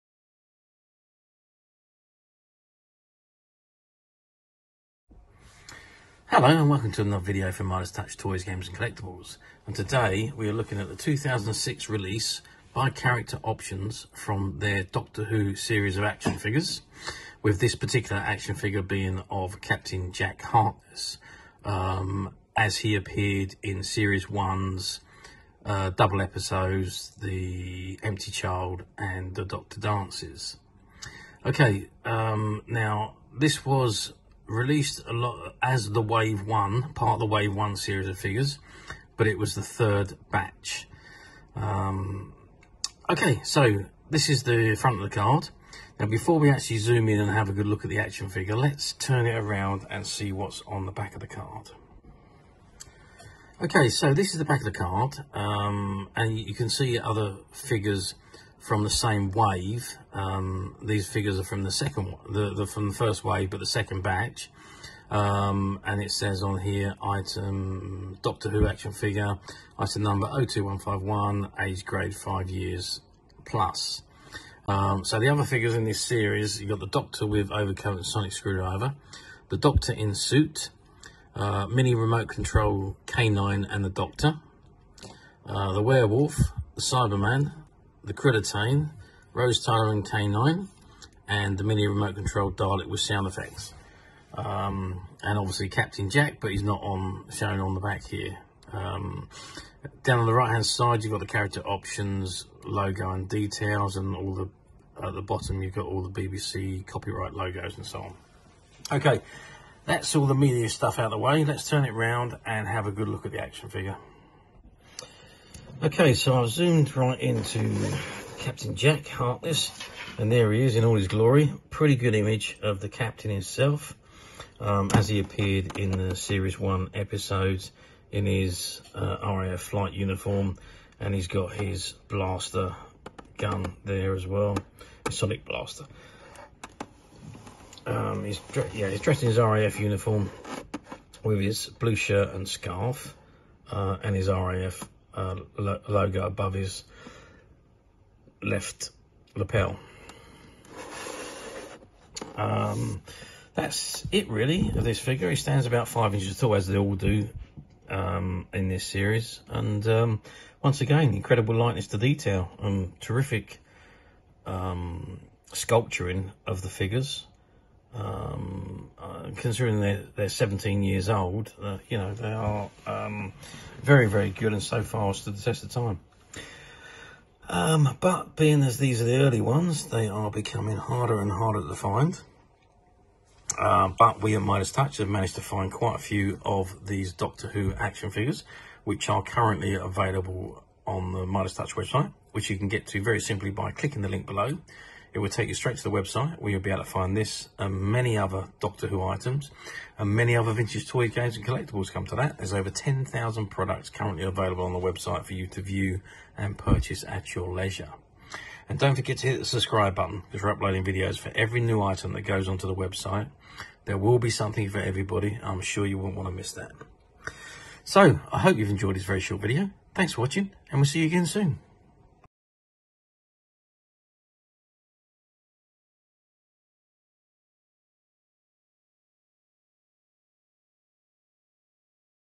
2005 Dr Doctor Who Captain Jack Harkness Highly Detailed Posable Action Figure
Vintage 2005 Dr Doctor Who Captain Jack Harkness Highly Detailed Poseable Action Figure - Brand New Factory Sealed Shop Stock Room Find. Released by Character Options in 2005 as part of their series of Doctor Who Action Figures. This fantastic Captain Jack Harkness Action Figure, stands 5 inches tall, he is highly detailed and fully poseable and looks incredibly like the on screen version as played by John Barrowman, and comes complete with his Sonic Blaster.